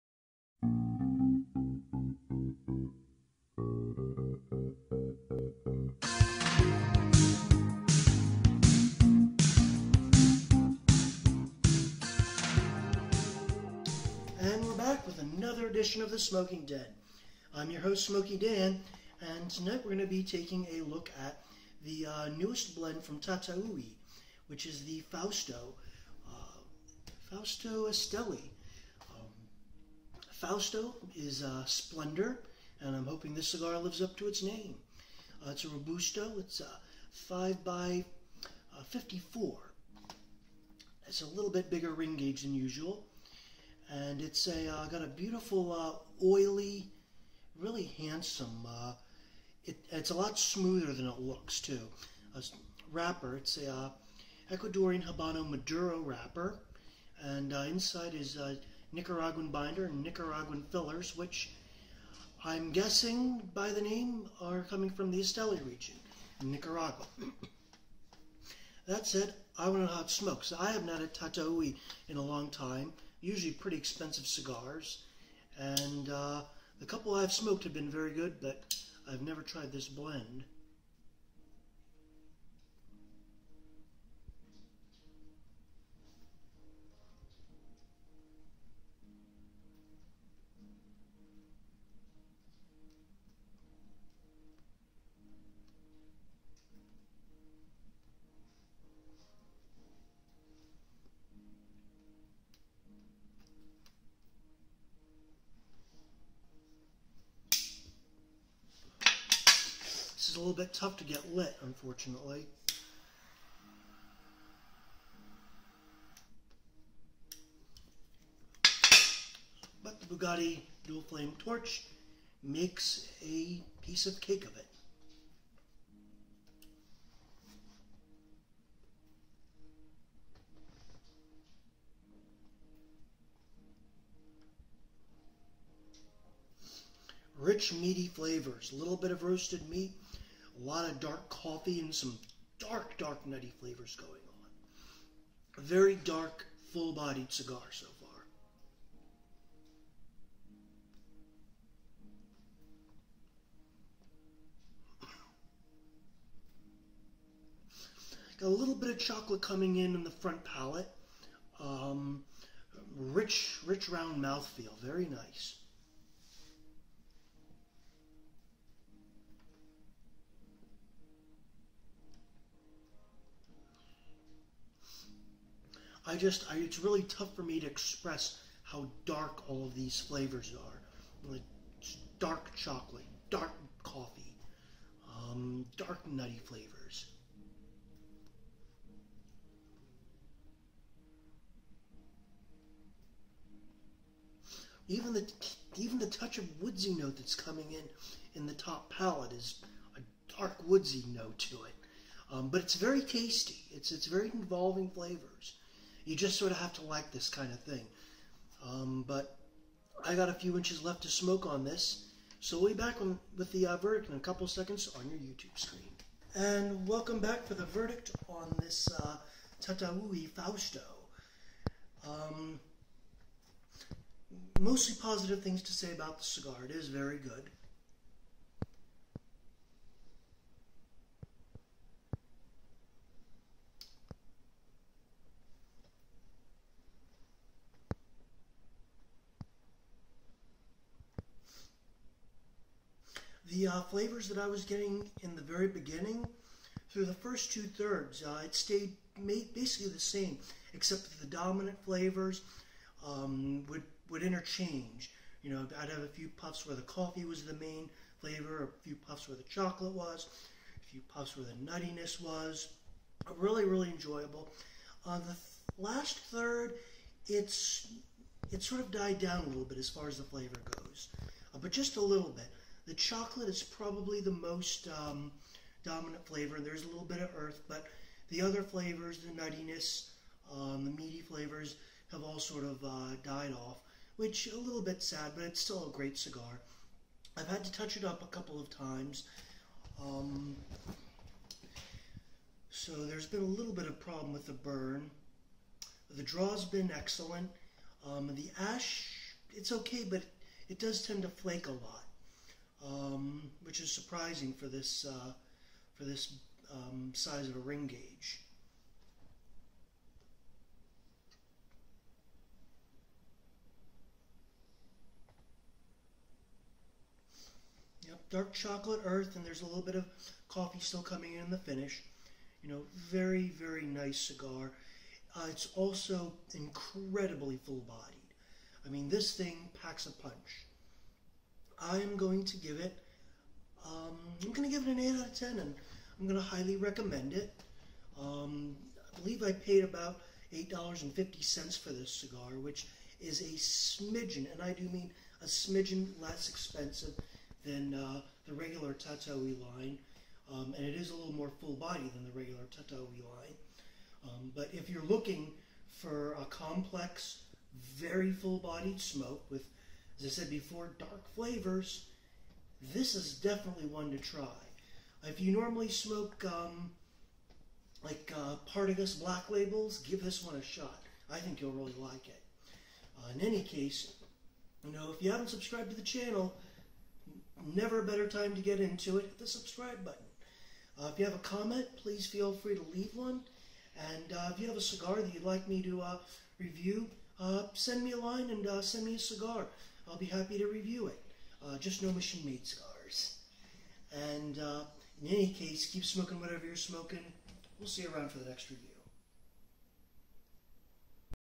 Tatuaje Fausto - Cigar Review
The Smoking Den reviews the latest from Tatuaje, the Fausto.  No bull, just a brief, honest cigar review.